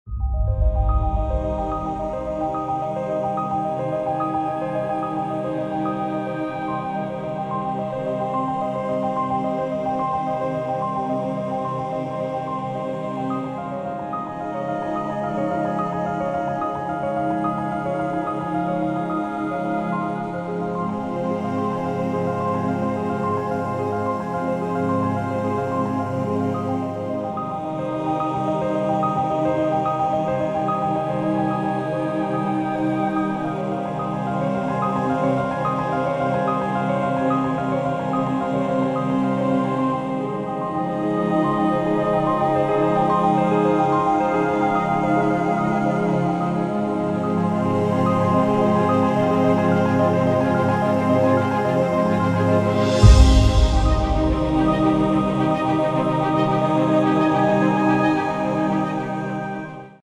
How Far is a Light Year? 🤯✨🔭📡
In this short, we take a Journey From Earth past the International Space Station and join Voyager 1 to the Reaches of Man-Kind. Then we proceed to Earth's Closest Star, Proxima Centauri and go Deeper into Space to Visit the Rosette Nebula (Caldwell 49)!!! This Mind Bending Video brings the Scale of Space into View. ~Clear Skies